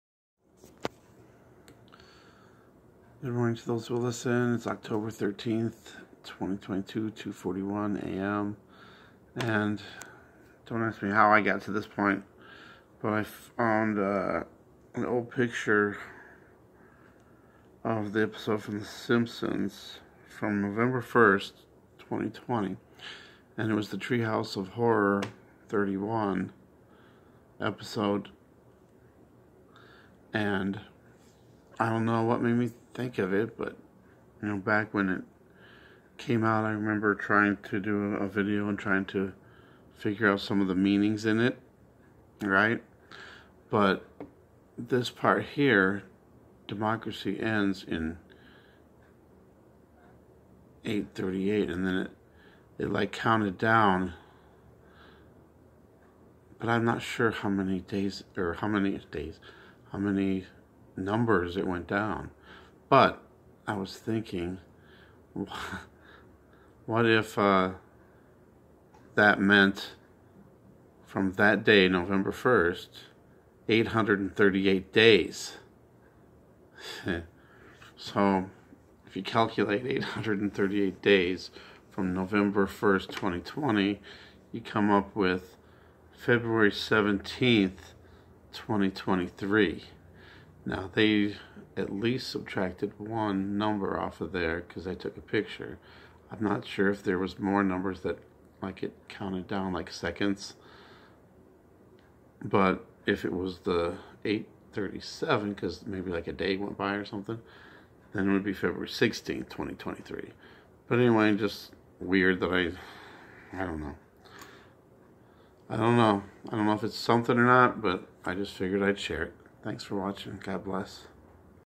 Is 838 Days the Hidden Message on the Simpson's??? October 13th 2022 2:41 am
I was looking through some old photos and found some pictures of The Simpsons Treehouse of Horror 31 from November 1st 2020 and put two and two together